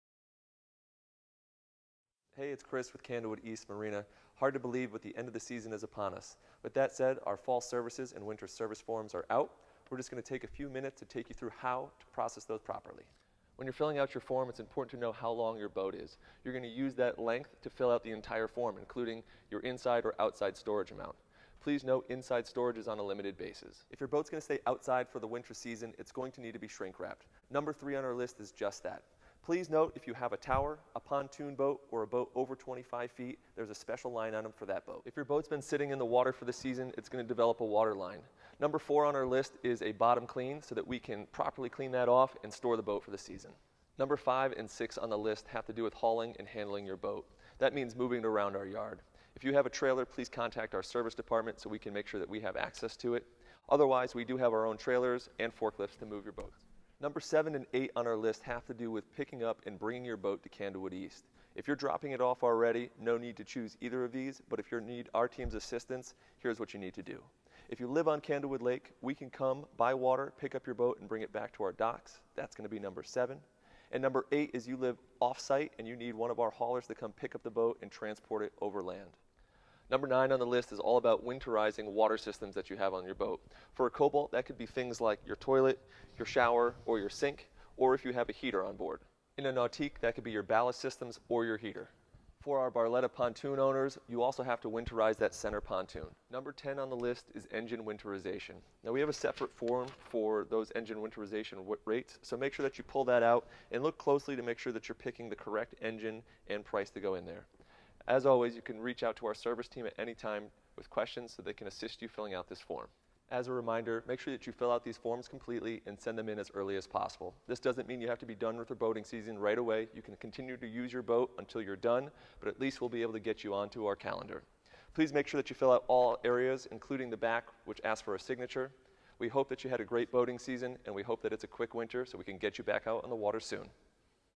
Boating Tips: Fall Services & Winter Storage Form
With Fall upon us the team here at Candlewood East Marina is ready to assist you with all of your fall and winter service needs. Our Fall Services & Storage Forms are now available on our